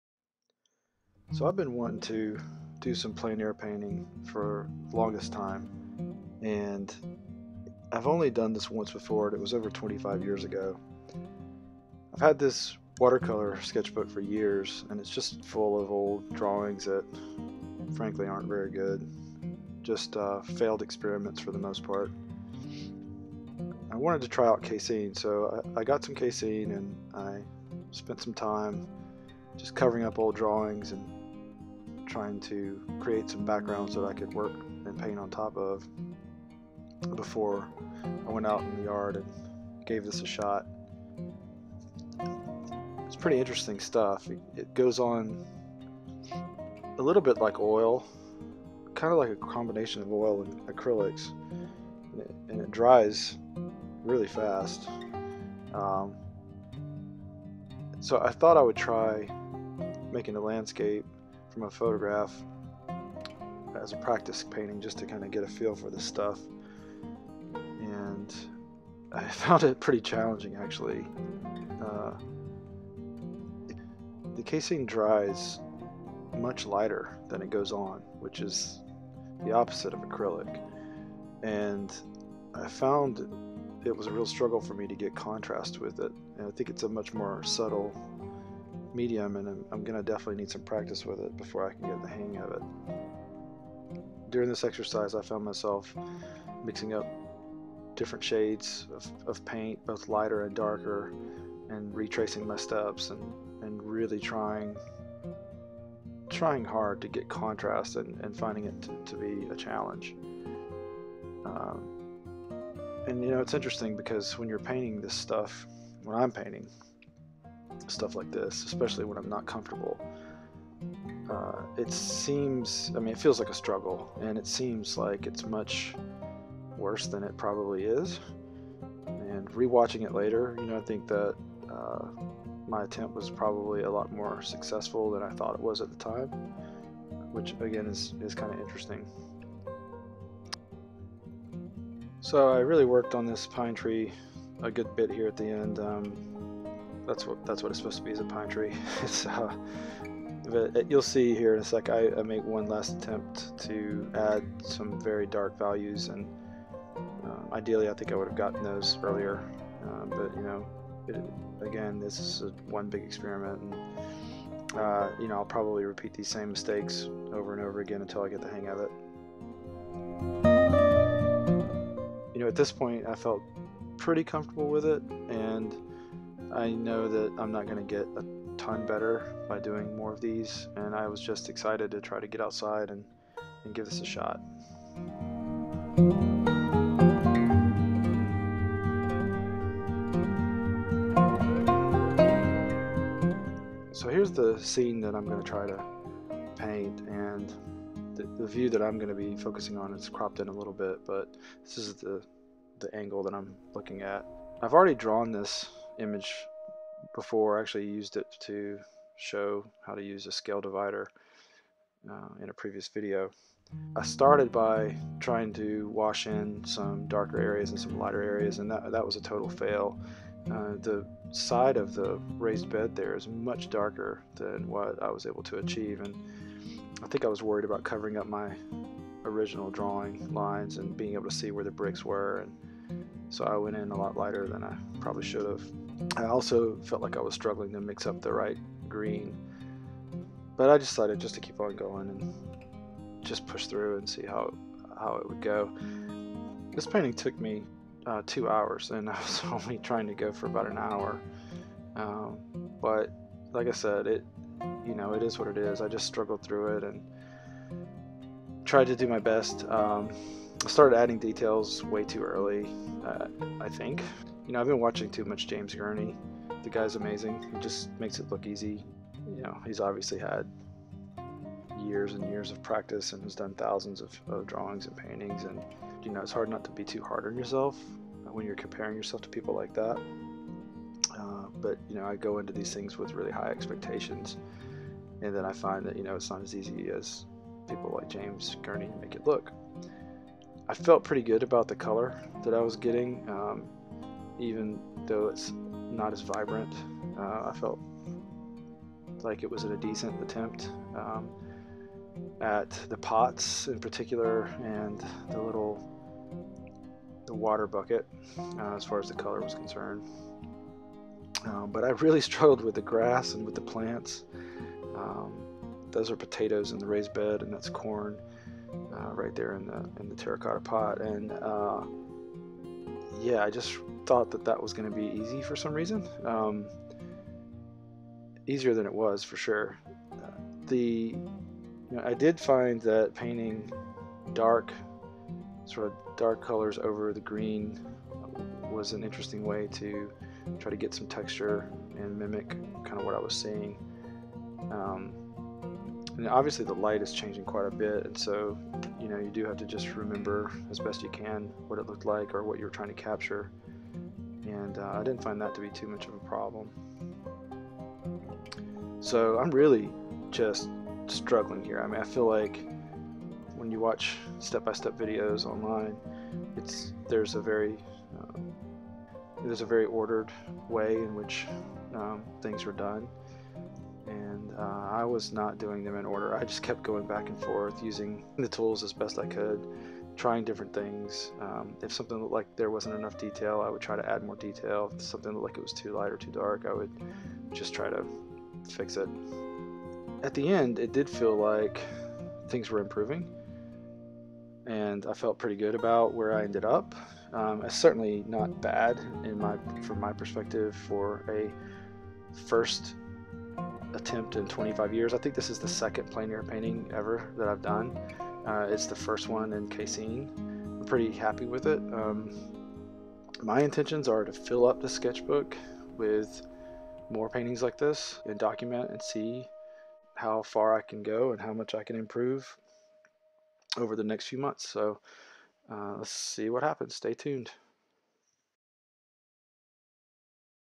My First "En Plein Air" Casein Painting in 25 Years - Watched too much James Gurney
En Plein Air #1: I finally get outdoors and try my first "En Plein Air" painting in over 25 years and my first one ever with casein. I started by painting some backgrounds and doing a practice landscape in an old watercolor sketchbook. I did really enjoy trying to learn a new medium and documenting a bit of our home as I captured our raised bed garden. I plan to paint a bunch more of these in the coming months to see just how much I can improve in this type of painting. Hope you enjoy and learn something!

Instagram Etsy